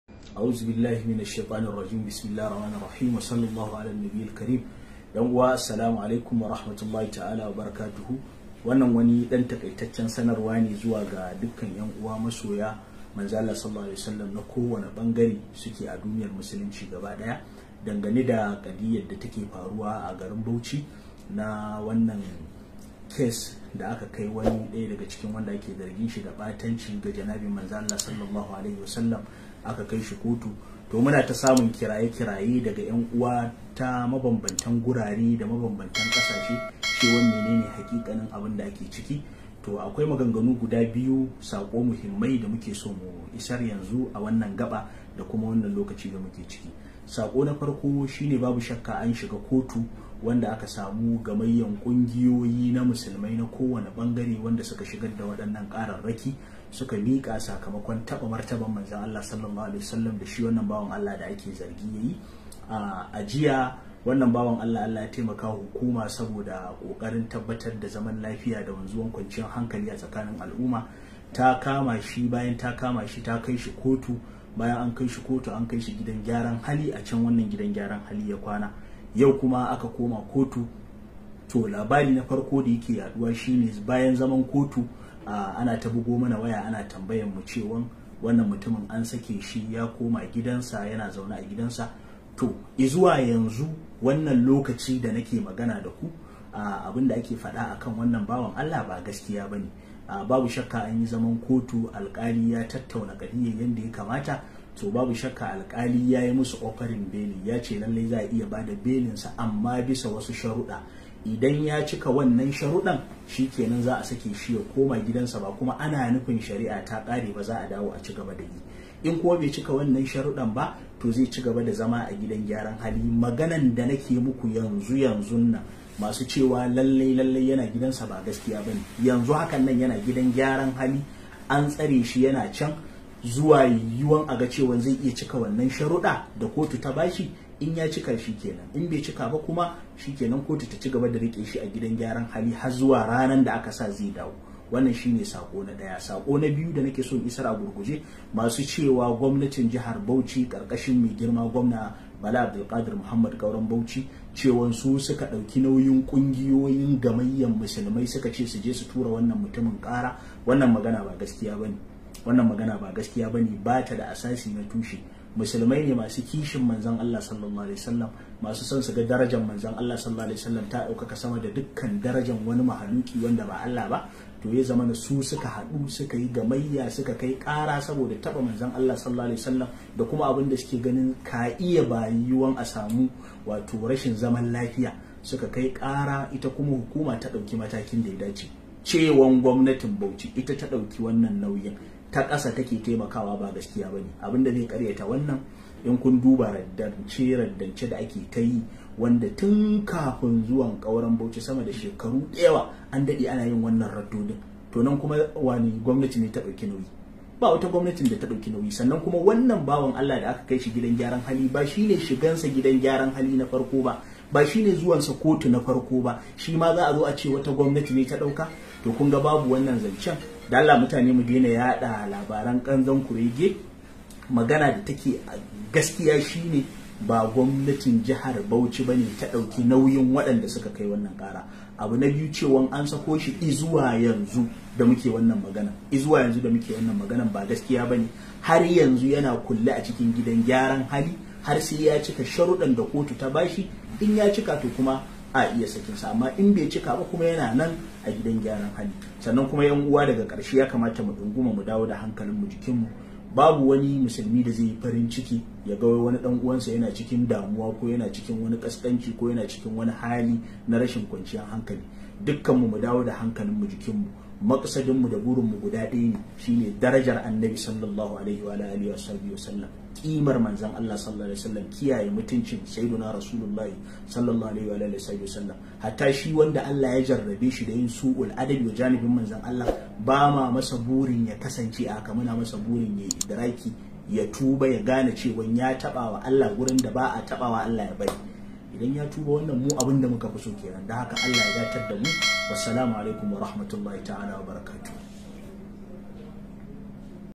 WATA SABUWA! BA A SAKI DR IDRIS BA YANA NAN A KULLE GA GASKIYAR MAGANA NAN/ SHEK ABULFATHI ATTIJJANY
Ba a saki Dr idris ba Yana nan a kulle ga gaskiyar maganan.

Mungode da ziyartar channel dinmu tare da fatan kuna amfana da kuma jindadin channel dinmu
MUNGODE!!! MUNGODE!!! MUNGODE!!!